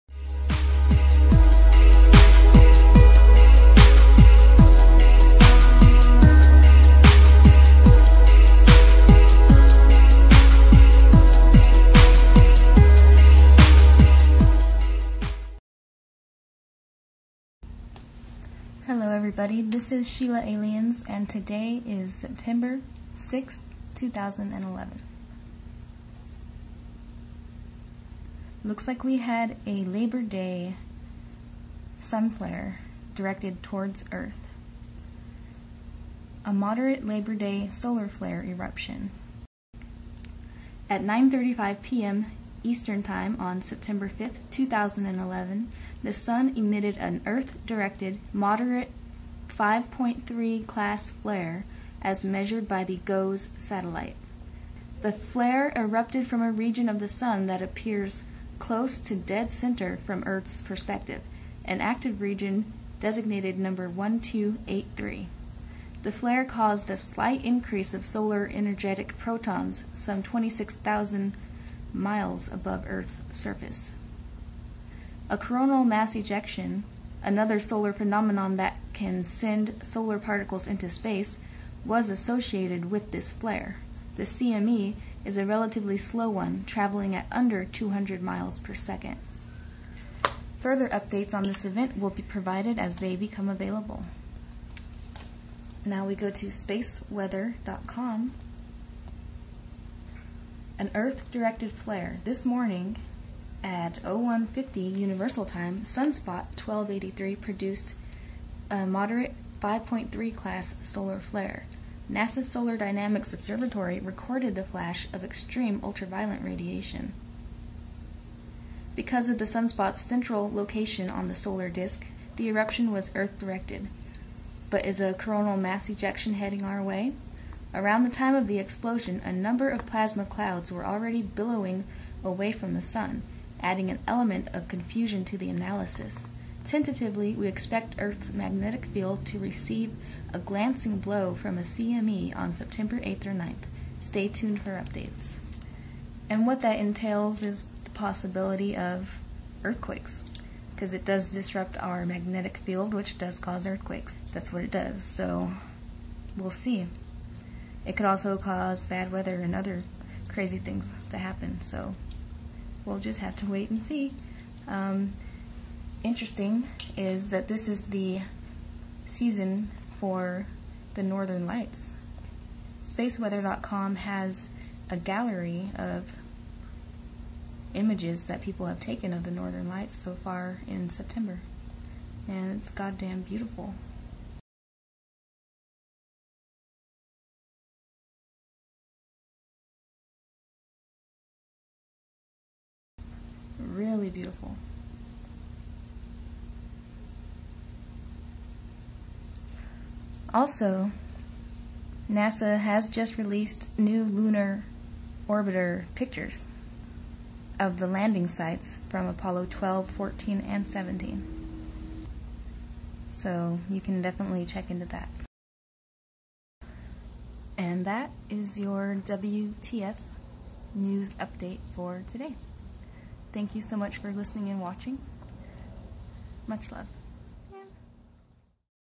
M5.3-class Flare - Earth-Directed, Possible Geomagnetic Storm September 8 - 9, 2011
UPDATE: X2-class Solar Flare Sends CME Hurling Towards Earth - September 6, 2011
"At 9:35 PM ET on September 5, 2011, the sun emitted an Earth-directed M5.3 class flare as measured by the GOES satellite. The flare erupted from a region of the sun that appears close to dead center from Earth's perspective, an active region designated number 1283. The flare caused a slight increase of solar energetic protons some 26,000 miles above Earth's surface."

"Because of the sunspot's central location on the solar disk, the eruption was Earth-directed--but is a CME heading our way? Around the time of the explosion, a number of plasma clouds were already billowing away from the sun, adding an element of confusion to the analysis. Tentatively, we expect Earth's magnetic field to receive a glancing blow from a CME on Sept. 8th or 9th.

WTF News... For the Win.